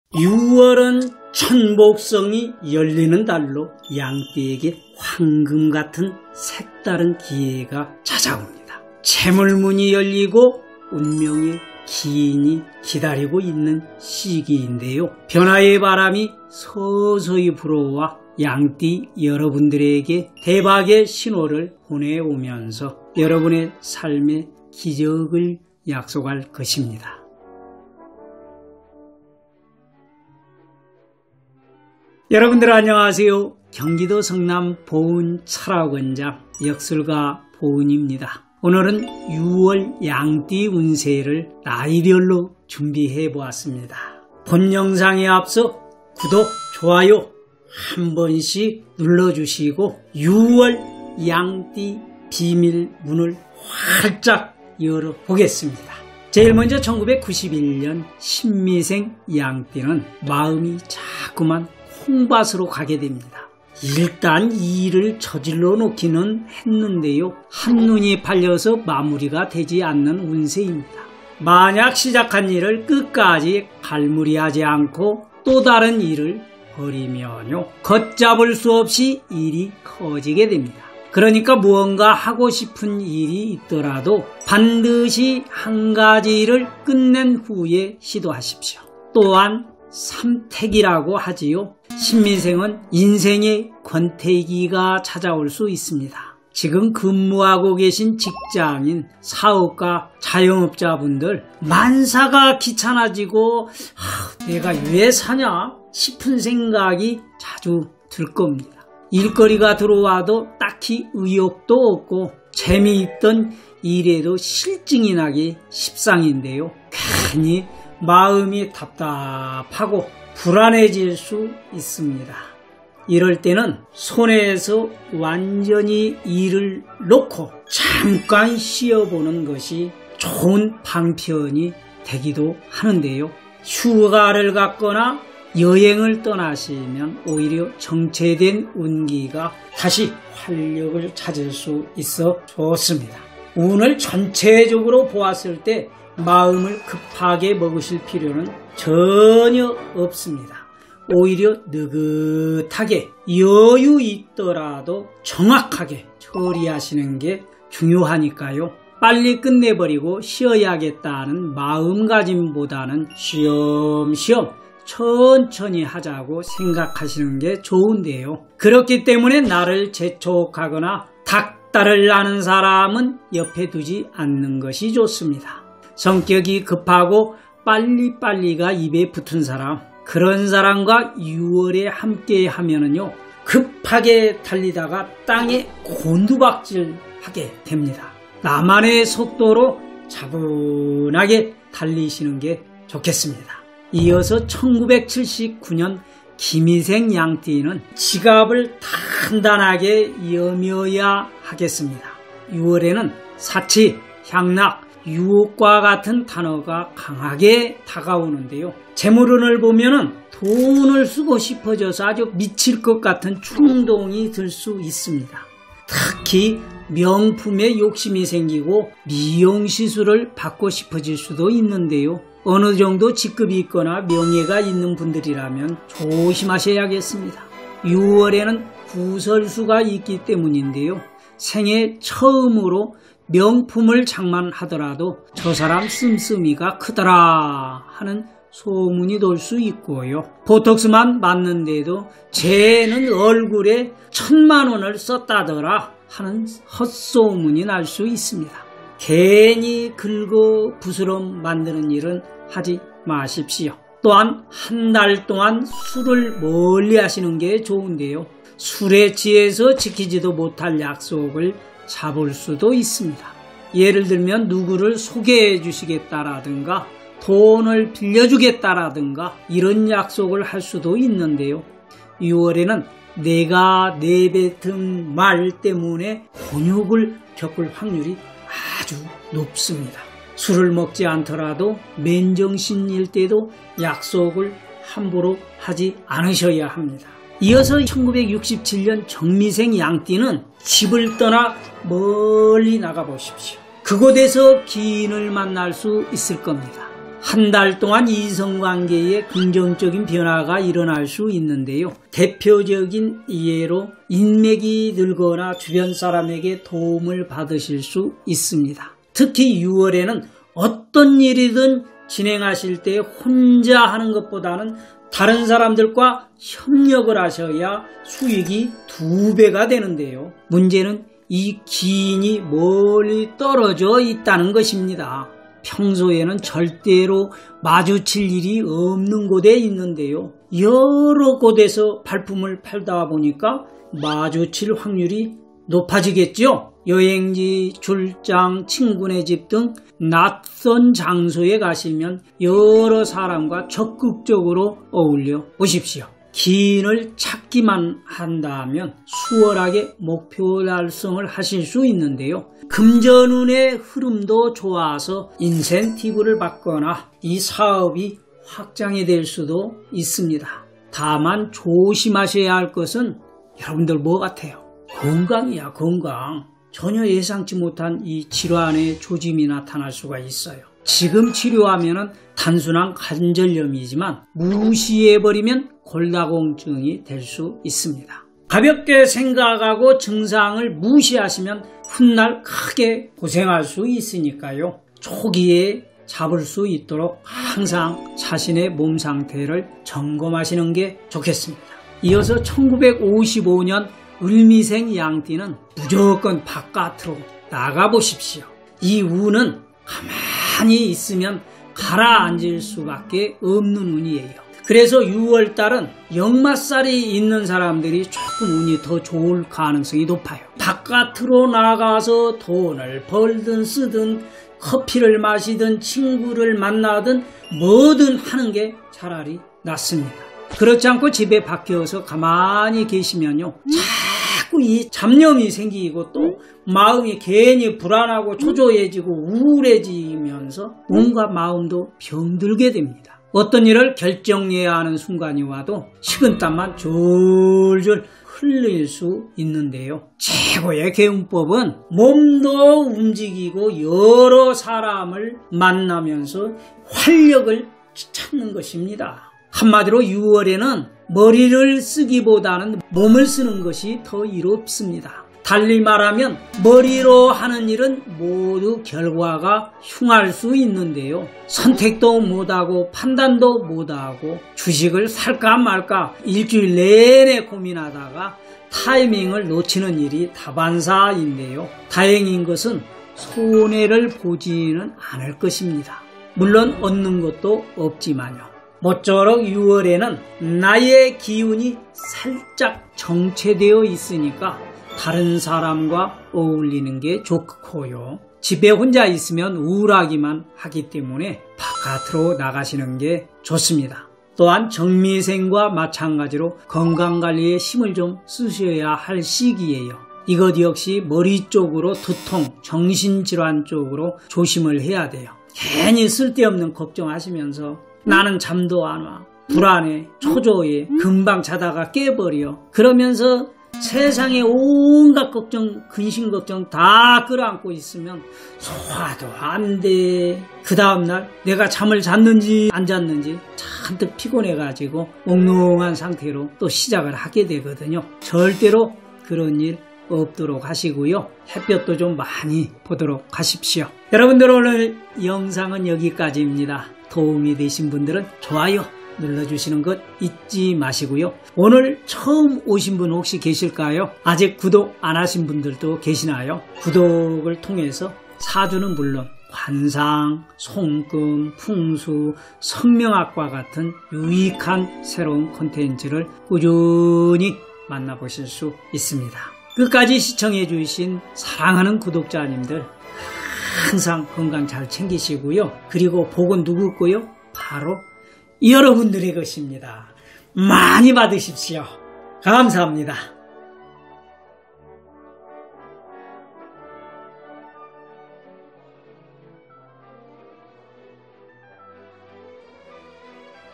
2024년 양력 6월 양띠운세/비밀의 천복성(天福星)이 열린다::황금키가 열려 귀인이 돕고 수익은 껑충!
◎6월 양띠운세는 천복성(天福星)이 열리는 달로 황금 같은 기회가 찾아온다
📌6월, 양띠 운세는 사주 팔자마다 그 구성과 발복의 시기는 제각각이니 그 해석 다릅니다.본 영상은 참고 삼아 흥미롭게 보시고 태클 대신 예의와 센스 있는 댓글도 함께 남겨주세요^^!
📌성남 분당 보은철학원 (예약문의- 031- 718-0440)
📌좀 더 많은 내용은 보은도사 블로그와 카페에서 보실 수 있습니다.